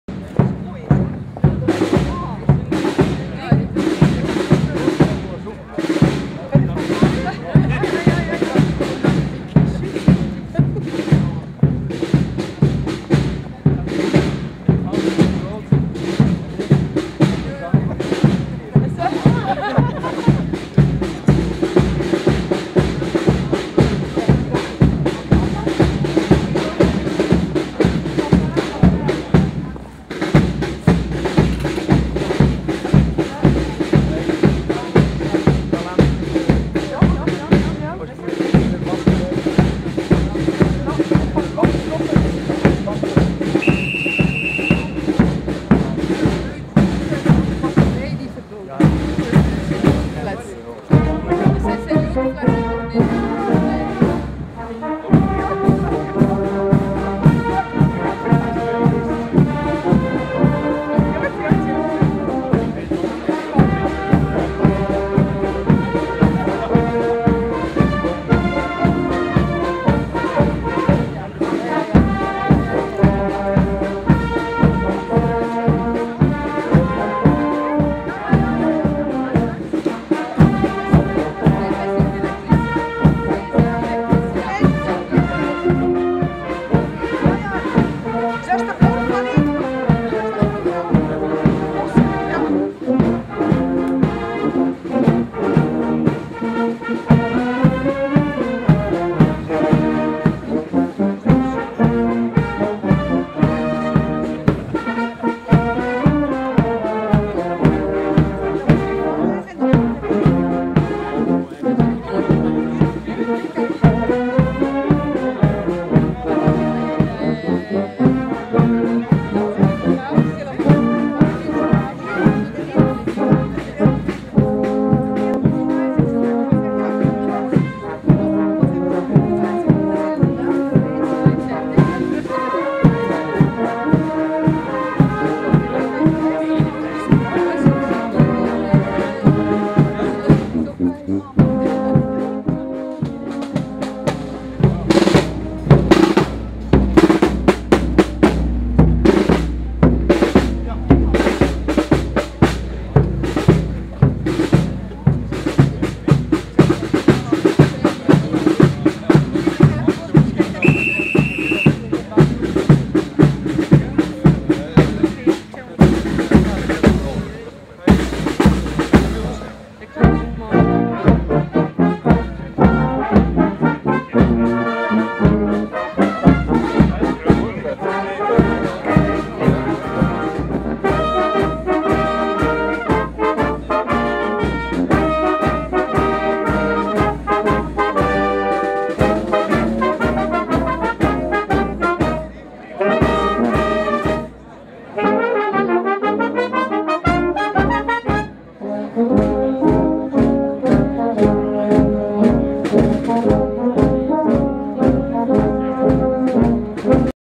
J17-KV-11018-3152 Adegem ontvangt kardinaal Jozef De Kesel./210
Feestelijke ontvangst van kardinaal Jozef De Kesel op zondag 11 juni 2017. De Koninklijke fanfare De Verenigde Vrienden Adegem begeleidt kardinaal Jozef De Kesel naar het Cultureel Centrum De Hoogen Pad voor de receptie en het ontmoetingsmoment met de kardinaal. (camera: Ignace Ryheul)